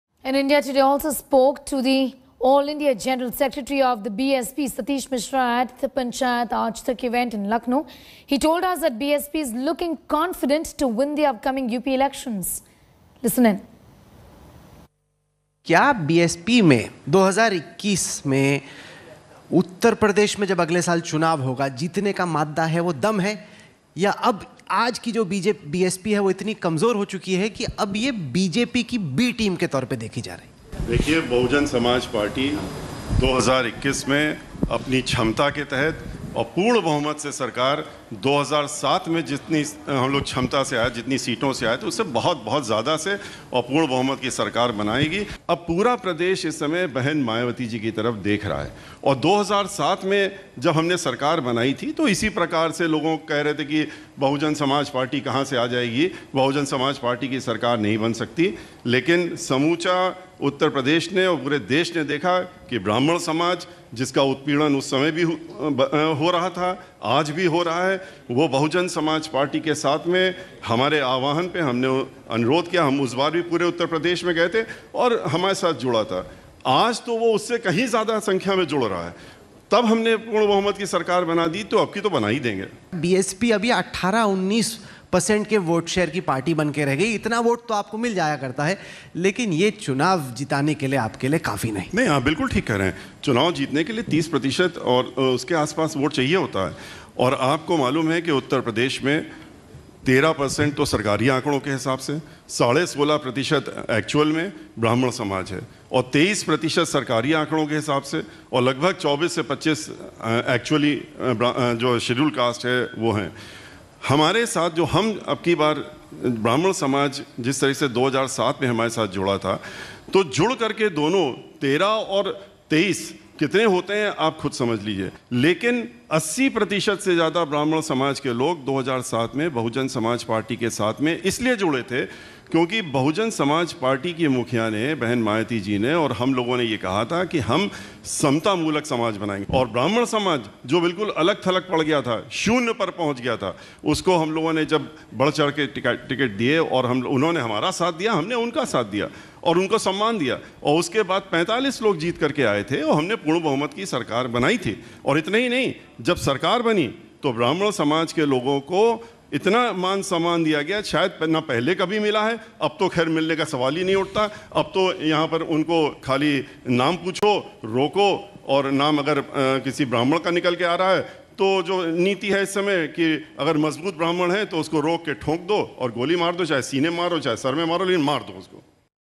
BSP Confident Of Winning Uttar Pradesh Polls Next Year, Satish Mishra At Panchayat Aaj Tak
BSP national general secretary Satish Chandra Mishra exuded confidence that his party will come to power in Uttar Pradesh next year with the support of the majority.